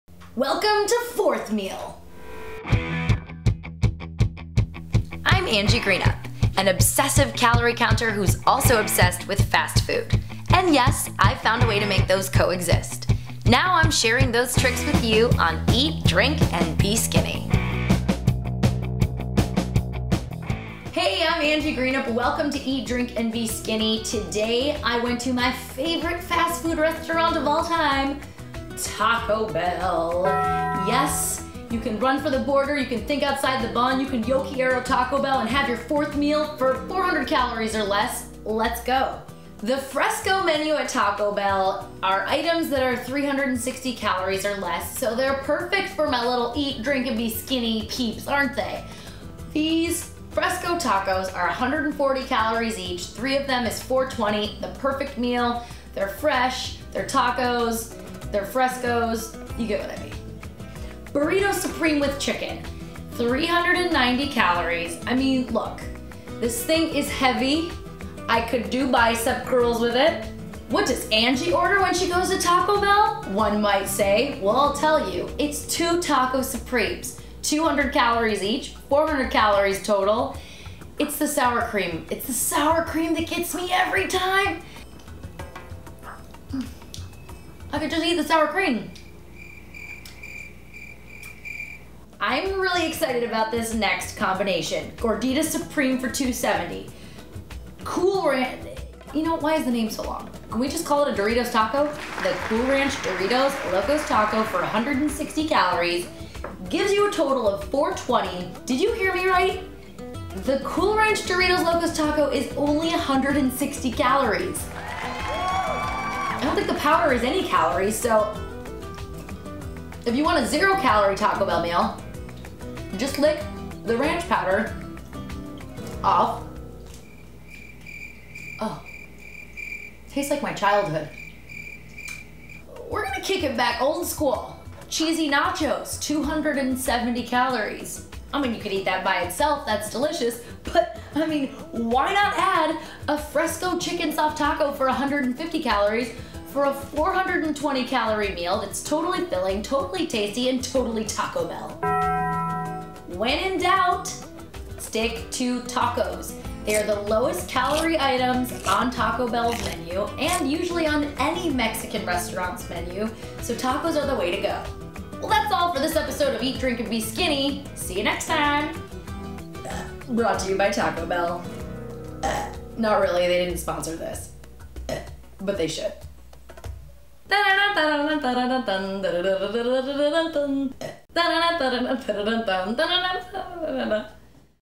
Taco Bell- Eat, Drink & Be Skinny with Angie Greenup
You don't have to give up eating at your favorite fast food restaurants. Whether you're on a diet, trying to lose weight, or just keeping your calories down-- you just have to know what to order! Learn the tricks Angie Greenup uses when she hits the drive thru!